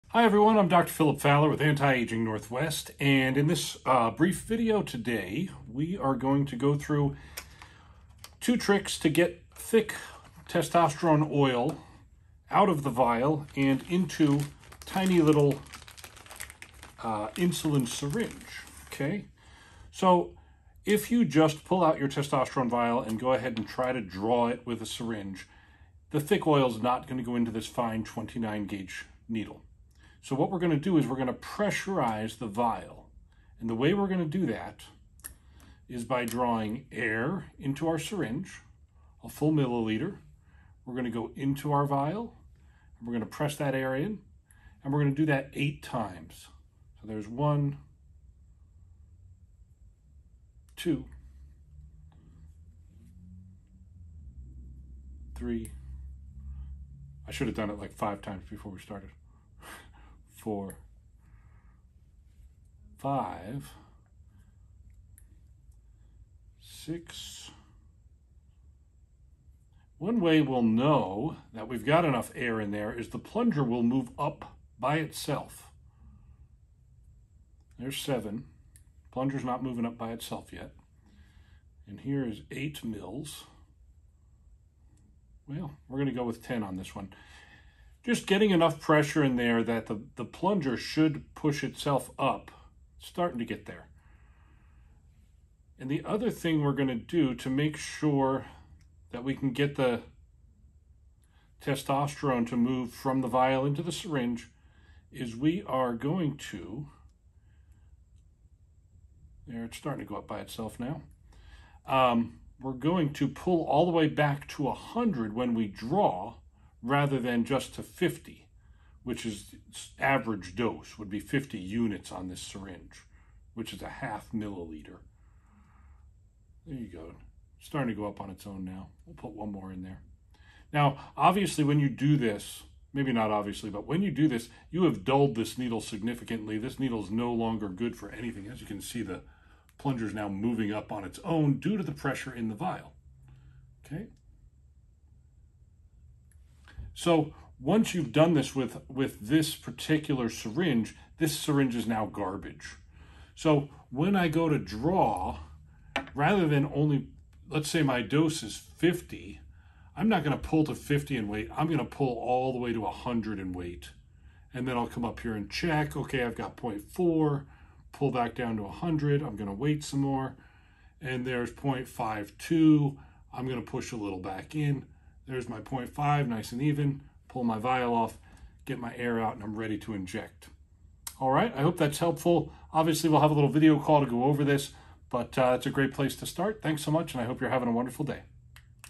How to Prepare a Testosterone Injection
Trouble preparing your testosterone injection? In this brief video, Dr. Faler shows you how to get the thick oil out of the vial and into the tiny syringe for your testosterone injection.

Learn how to pressurize the vial and prepare your testosterone injection quickly and easily.

Having low testosterone can have consequences for your health, regardless if you are a man or a woman. For example, if you have low testosterone levels, you might suffer from low muscle mass, low libido, and even low bone density.

Fortunately, testosterone replacement therapy (TRT) might be the solution to this problem.